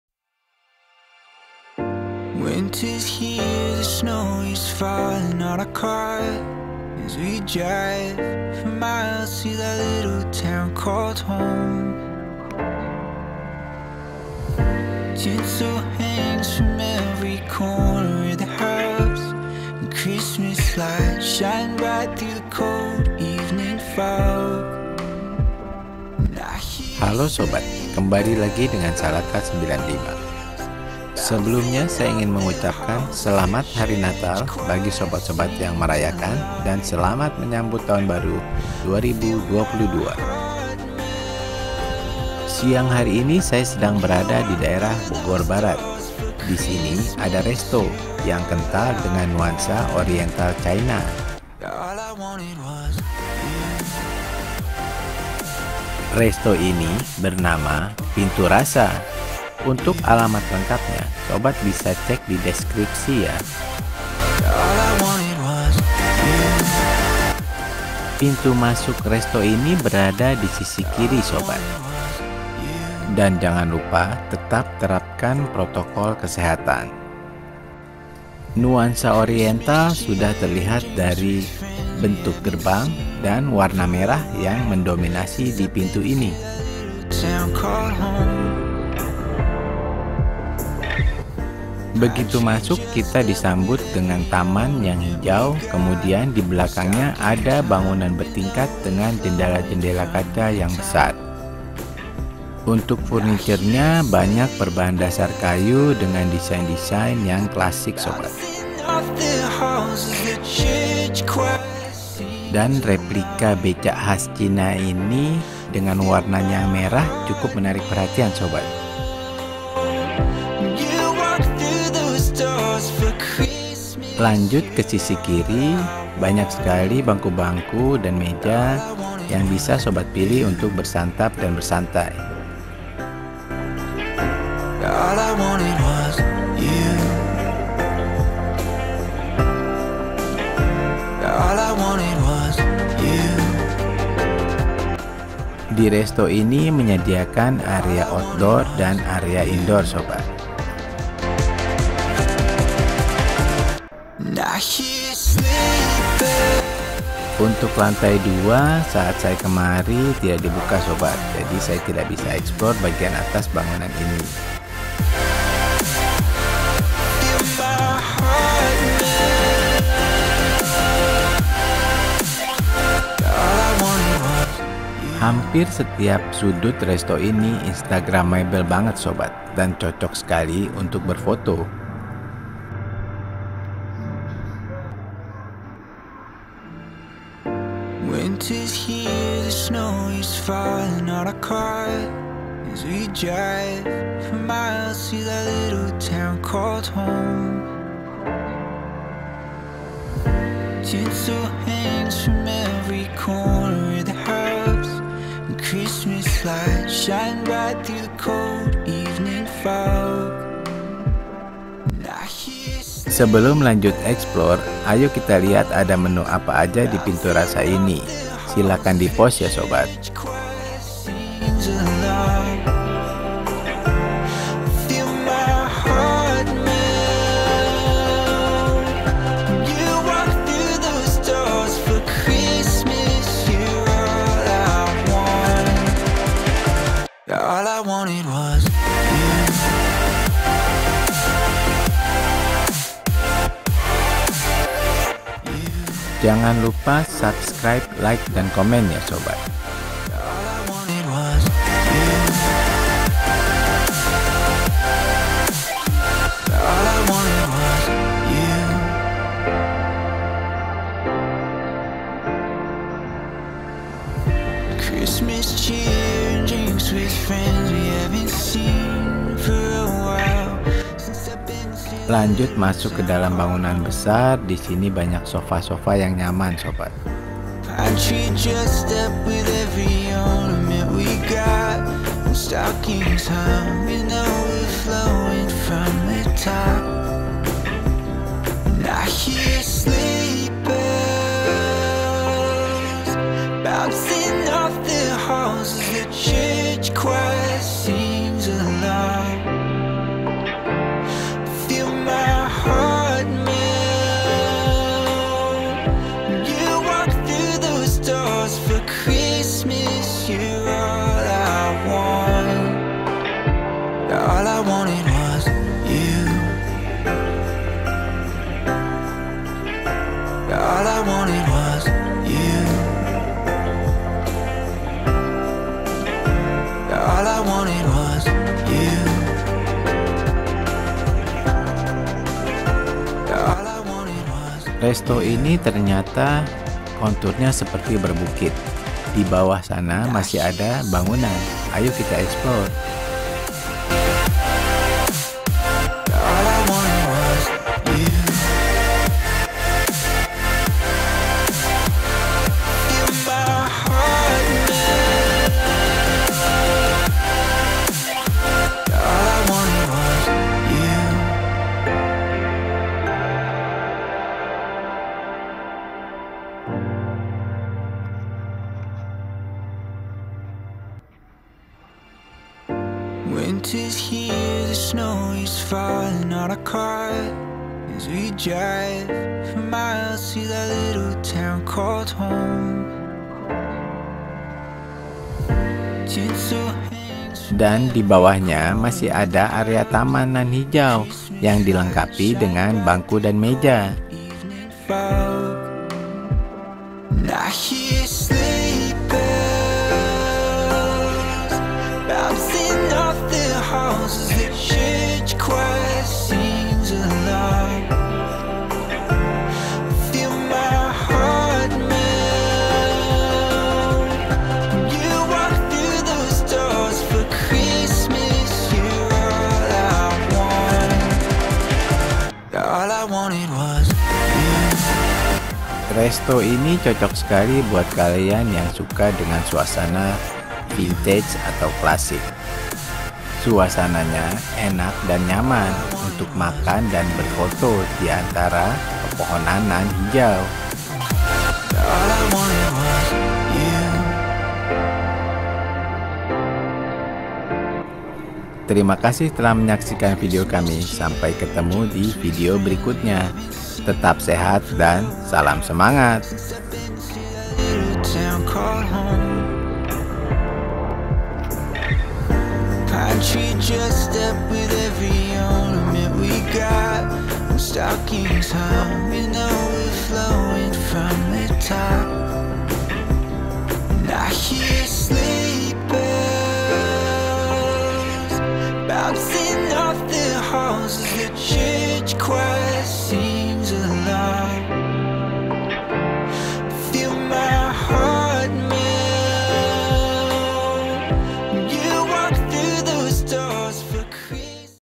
PINTU RASA BOGOR Resto Suasana Vintage Oriental Keren Adem Instagramable
Cafe Pintu Rasa berlokasi di Jl. Cilubang Mekar No.8, RT.01/RW.08, Situgede, Kec. Bogor Barat, Kota Bogor, Jawa Barat 16115, Indonesia.
Berada di antara pepohonan rindang, suasana vintage ala oriental yang kental hadir di sini, di tambah furniture kayu classic, asik buat menikmati makanan dan minuman.